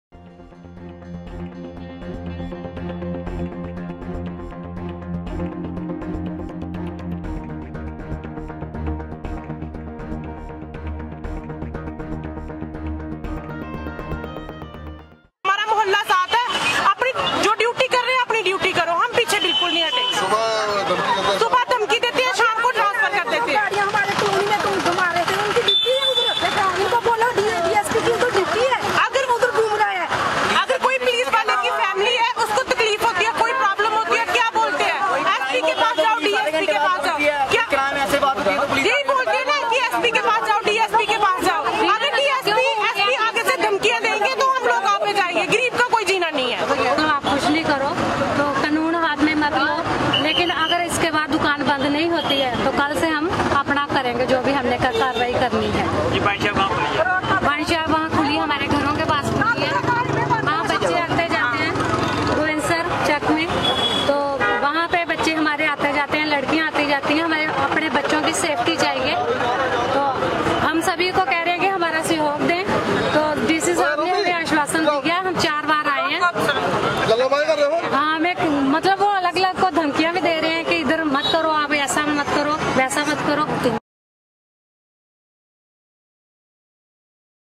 Kathua: Women protest against opening of wine shop
| Excelsior News |